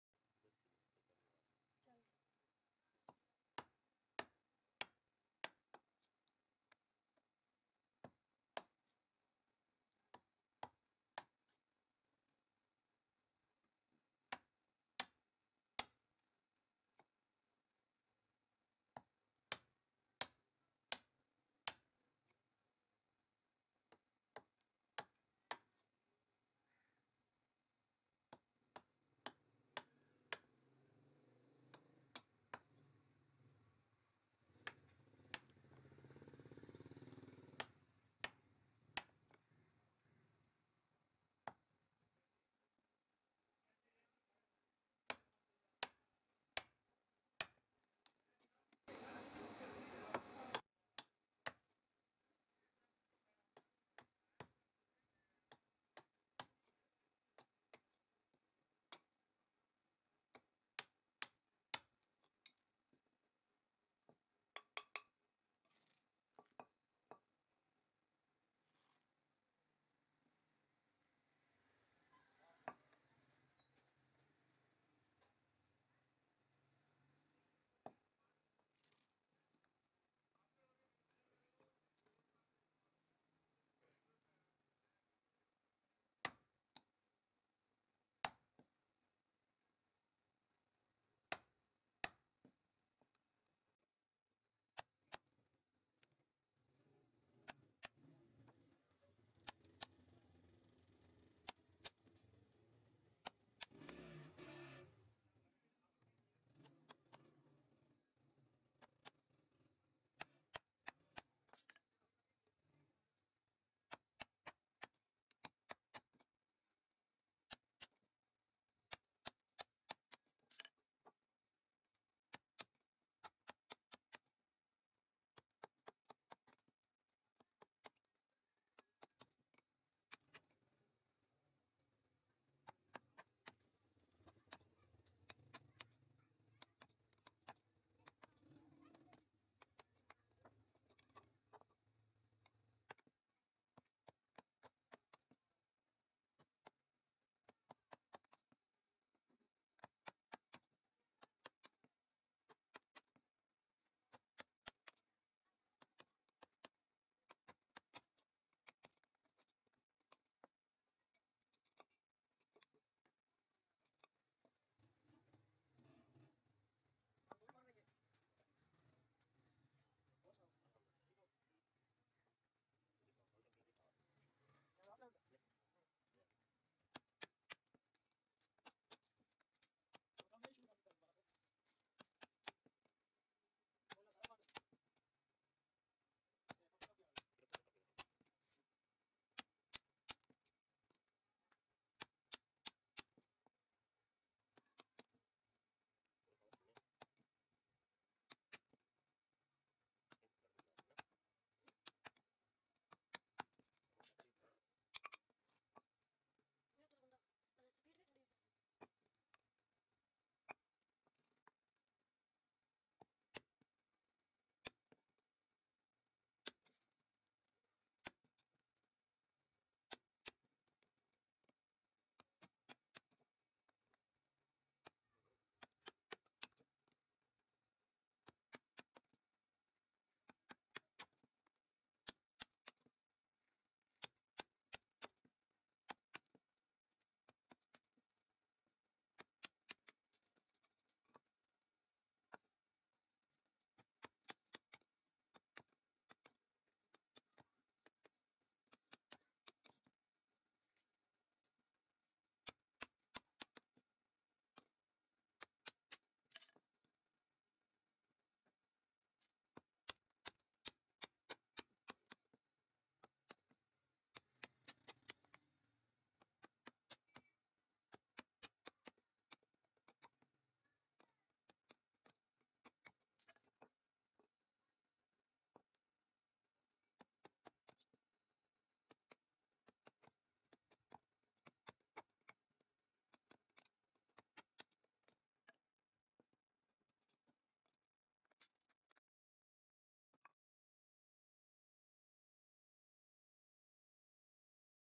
How does a villager make a crop hole |the skills
How does a villager make a crop hole
Total hand made. If you like the the video, please comment. thank you!